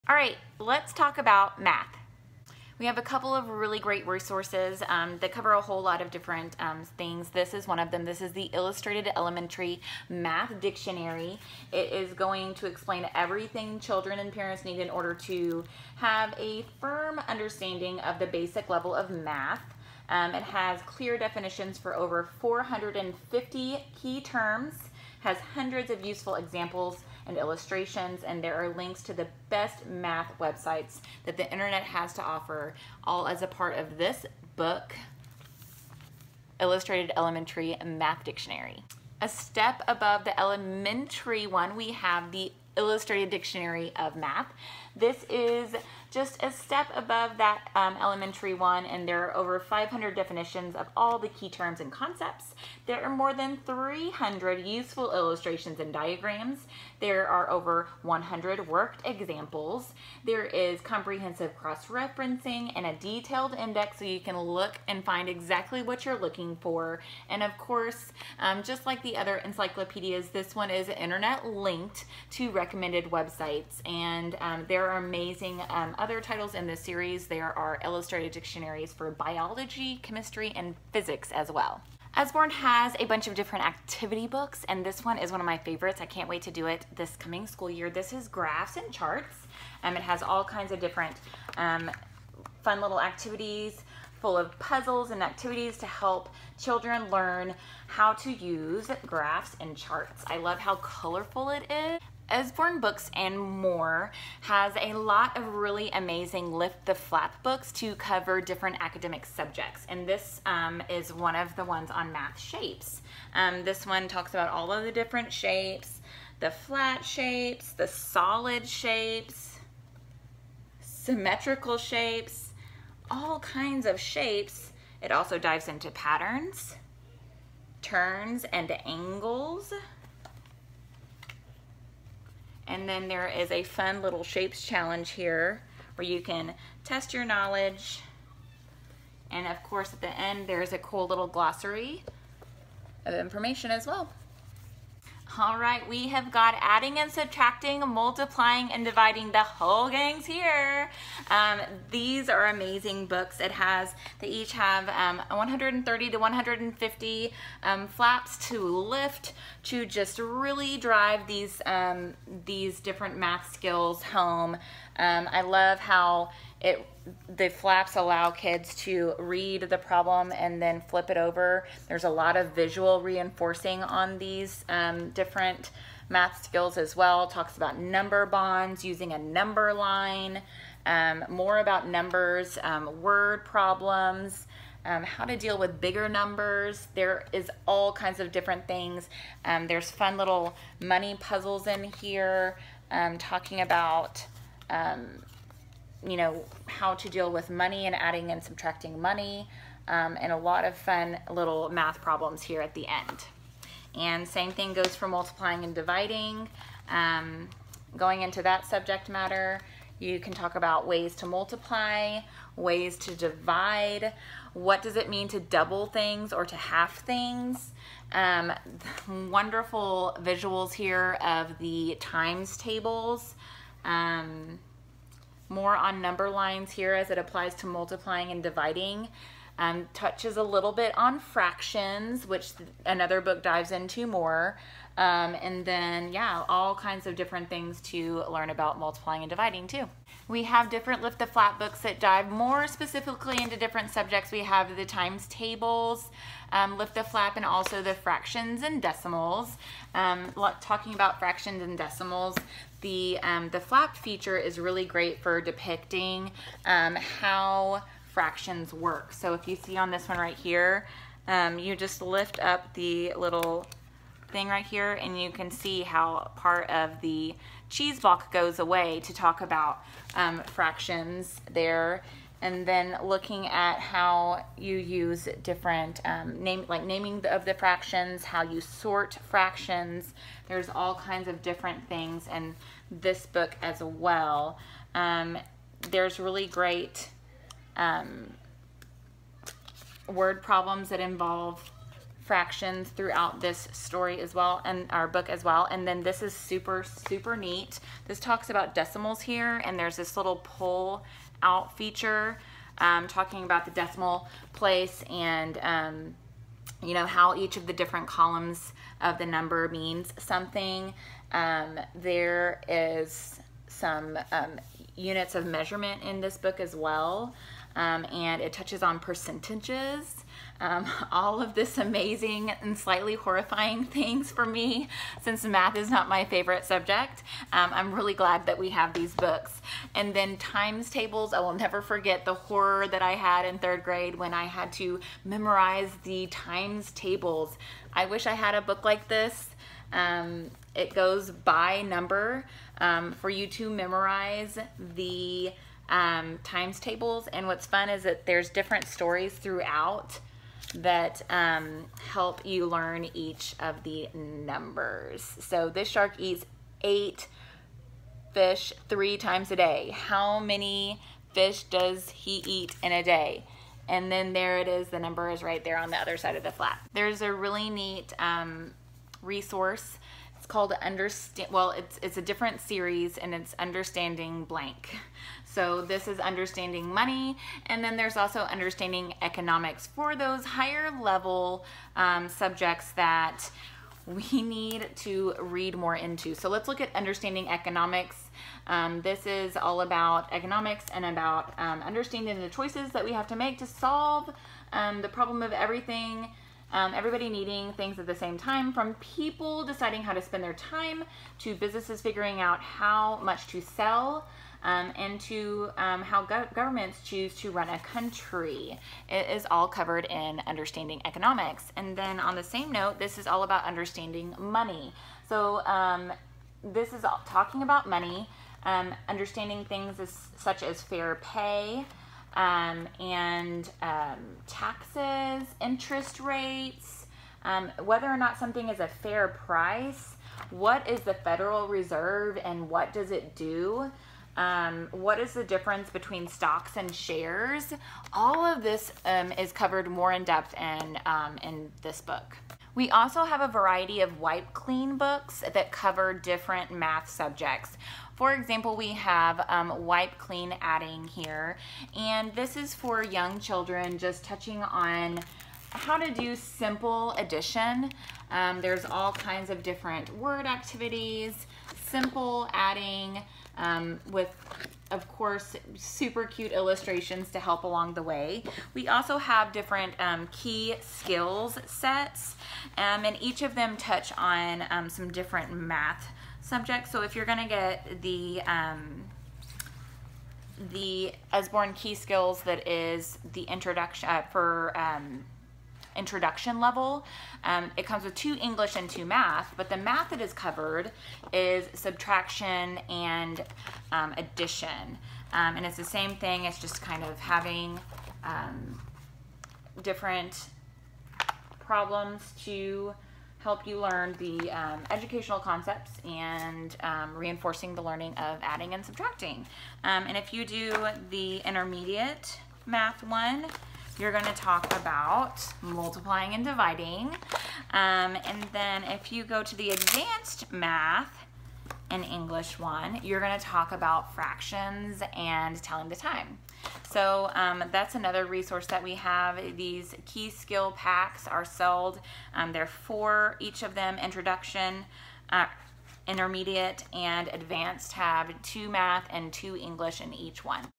MATH- Learning Resources from Usborne Books & More Fall 2020
Whether you are learning from home or with a teacher, Usborne Books & More has the perfect learning resources to support your educational experience involving MATH!